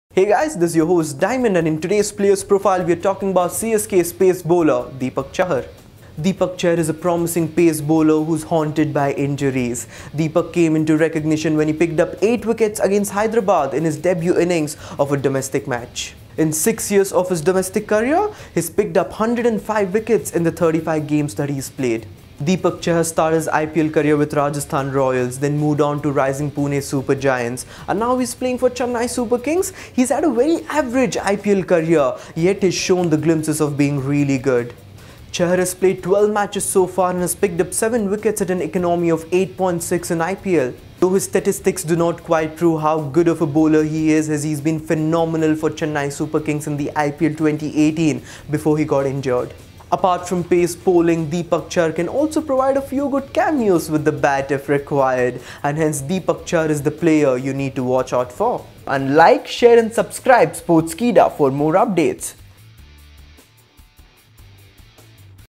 Player Profile: Deepak Chahar | IPL 2018 | Sportskeeda
Deepak Lokandersingh Chahar (born 7 August 1992) is an Indian first-class cricketer who plays for Rajasthan in domestic cricket. He is a right-arm medium-pace bowler who is currently a part of Chennai Super Kings squad in the Indian Premier League.